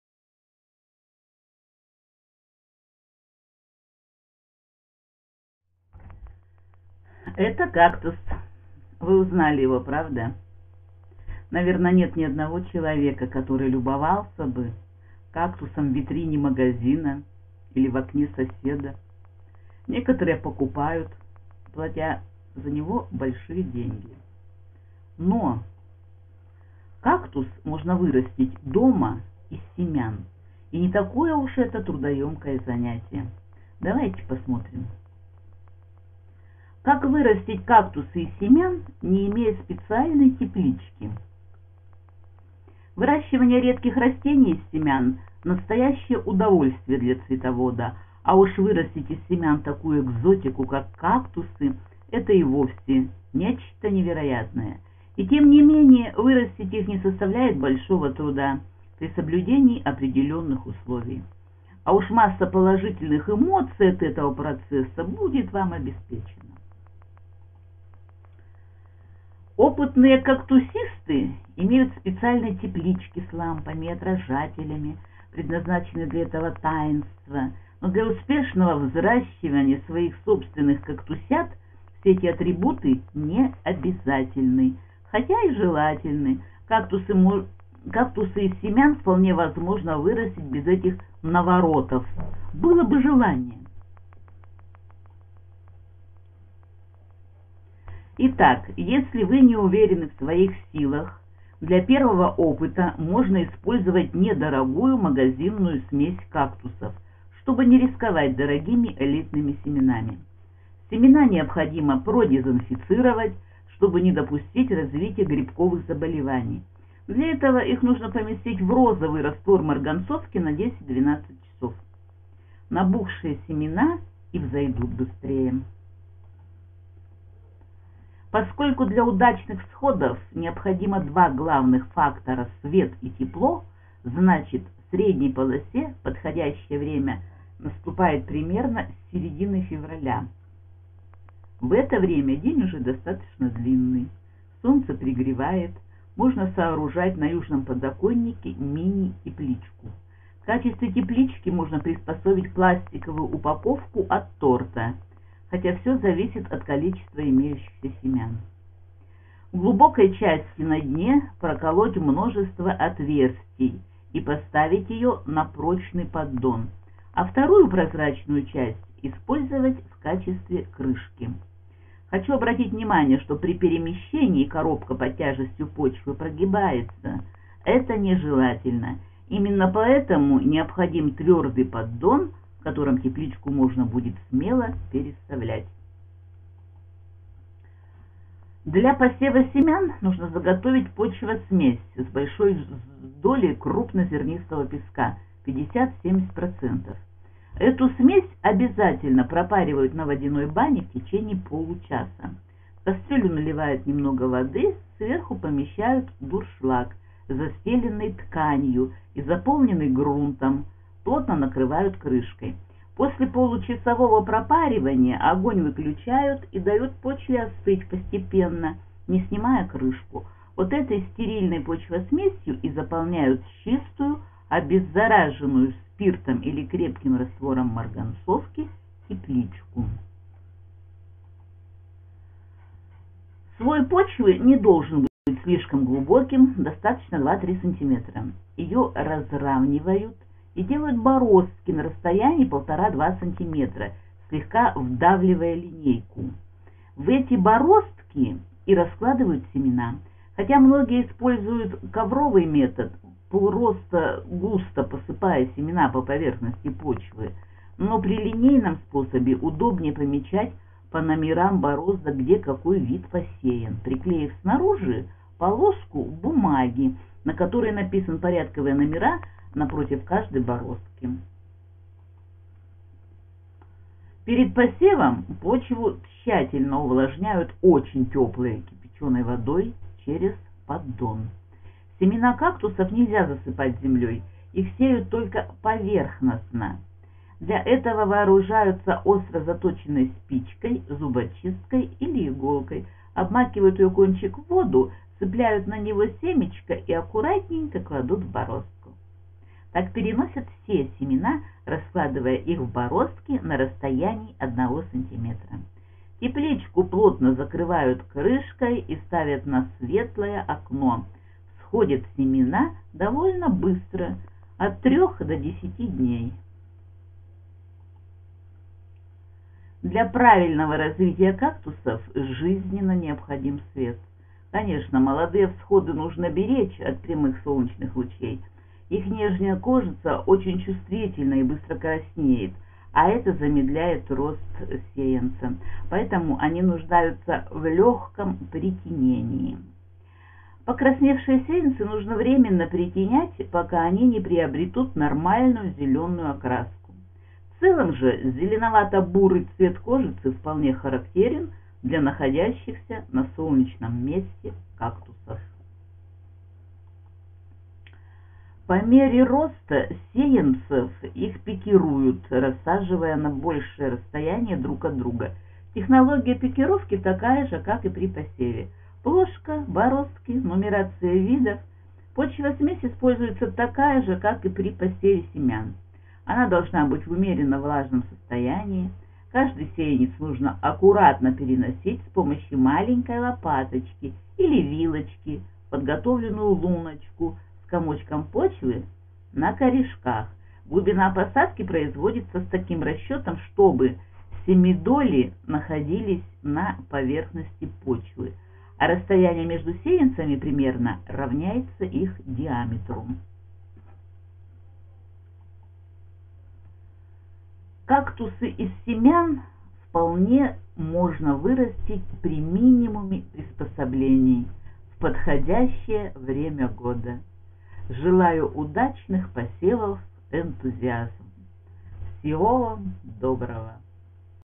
Кактусы из семян
Выращивание редких растений из семян – настоящее удовольствие для цветовода. А уж вырастить из семян такую экзотику как кактусы – это и вовсе нечто невероятное. И, тем не менее, вырастить их не составляет особого труда при соблюдении определенных условий. А уж масса положительных эмоций от этого процесса будет вам обеспечена!